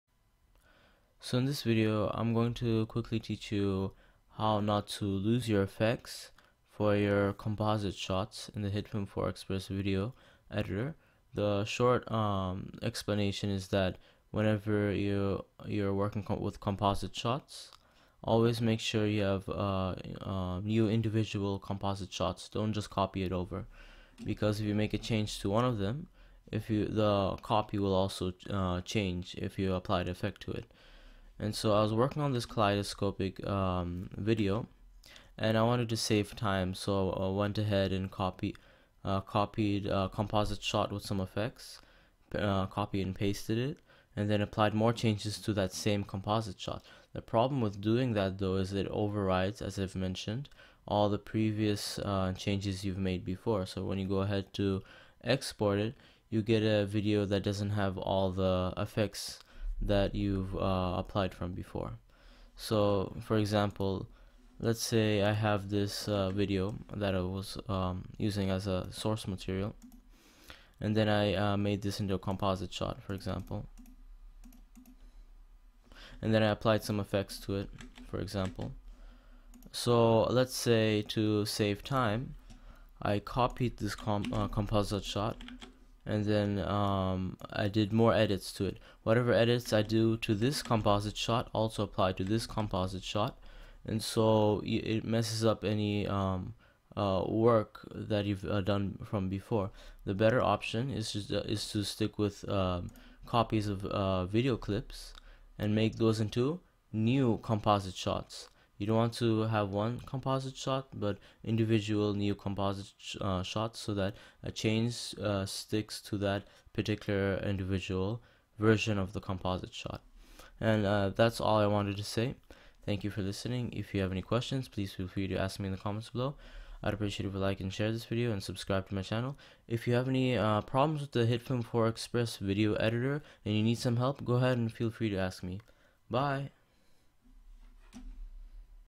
How not to Lose Changes & Effects in Composite Shot in Hitfilm 4 Express
In this video, I outline a piece of advice that will prevent you headache in the future, if you work with hitfilm 4 express. Basically, you should never copy a composition or composite shot. Because if you make changes to that copy, then those same changes will apply to the original composite shot, & vis versa. So to prevent this, NEVER copy composite shots. Instead, you should copy over effects from one composite shot to the other new unique composite shot.